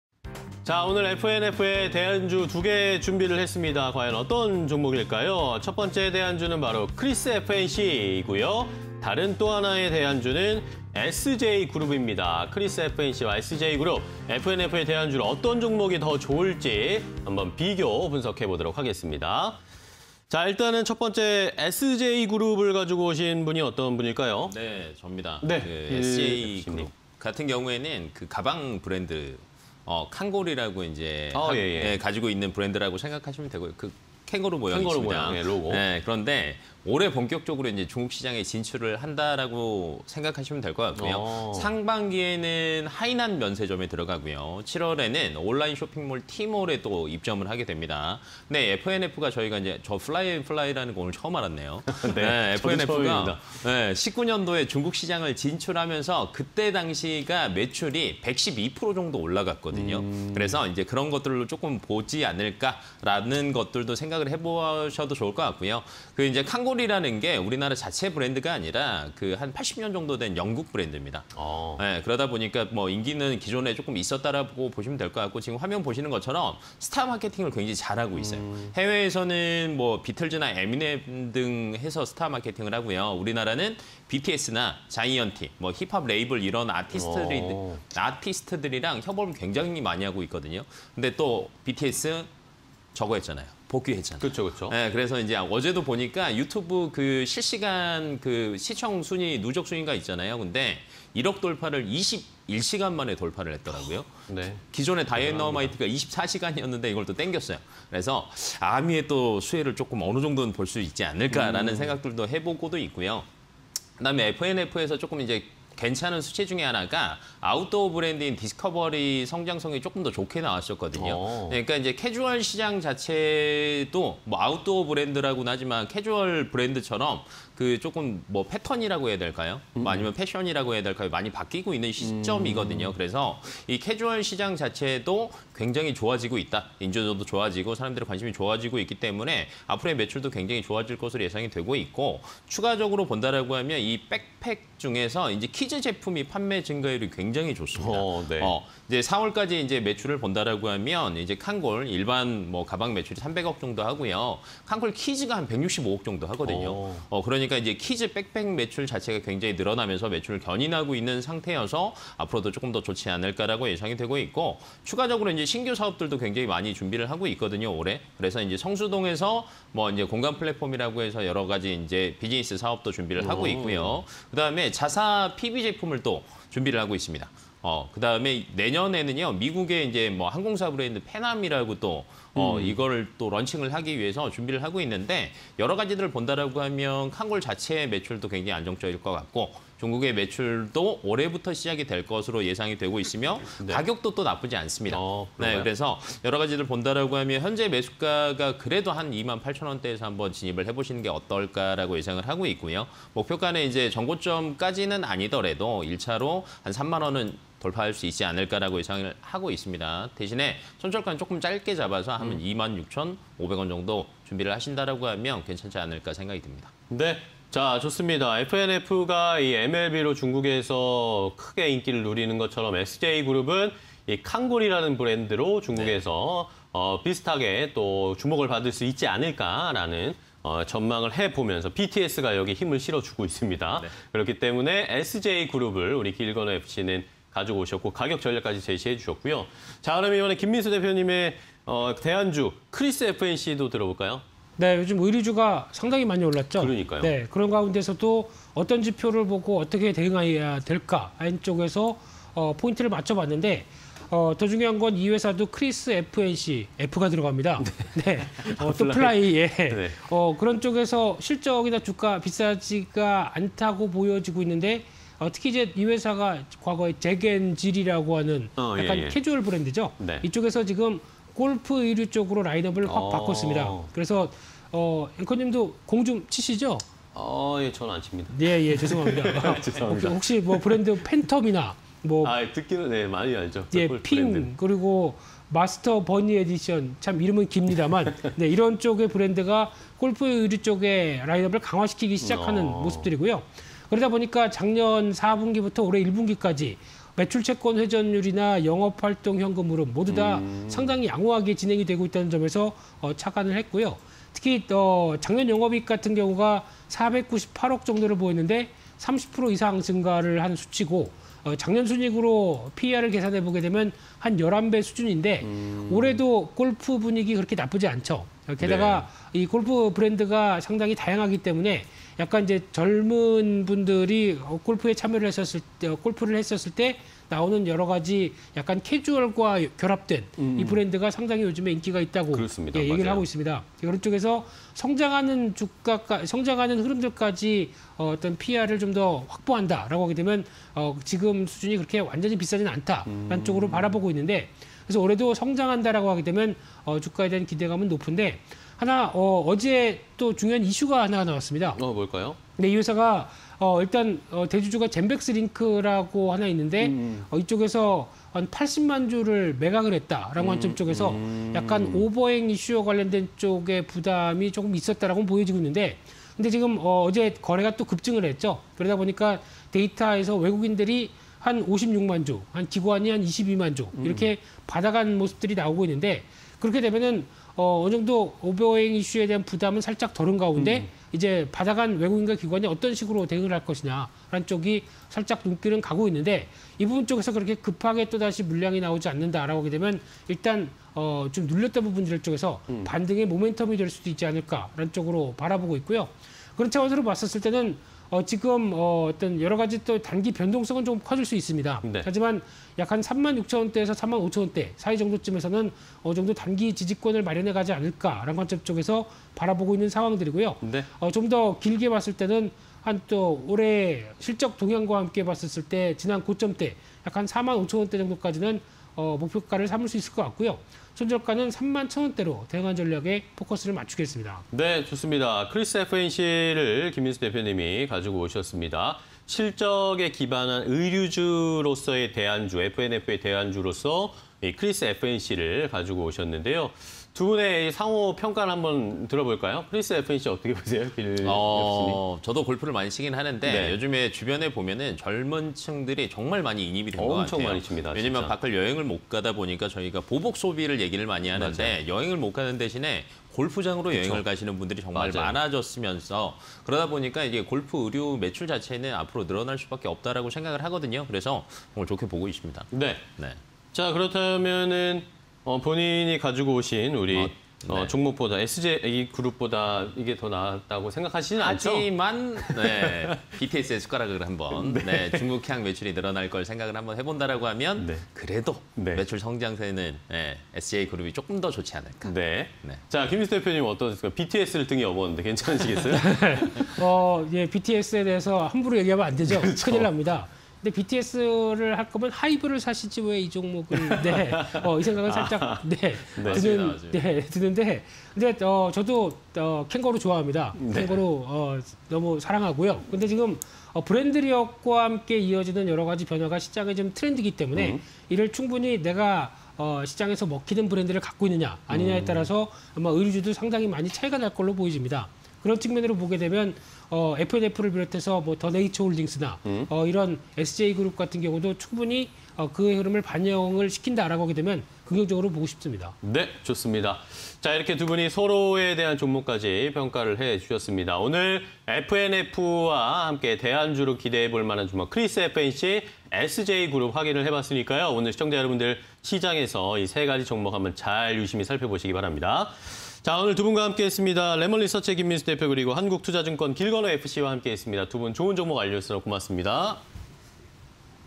에스제이그룹 VS 크리스에프앤씨_대안주를 찾아라 (20210525)
- 본격적으로 중국시장 진출
- 2019년 중국시장 진출 후 매출 112% 증가
- 영국 캐주얼 브랜드 ‘캉골’ 도입
- 캐주얼 시장에서 브랜드 인지도 확보
- 보복소비로 제품 판매 증가
- 신규사업 준비 이상무
- 美 최초 항공사 브랜드 ‘팬암’ 라이센스 계약

- 비싸지 않은 주가 매력 보유
- 골프의류 라인업 강화
- 매출채권 회전율 및 현금흐름 호조
- 올해 기저효과 중심 성장 기대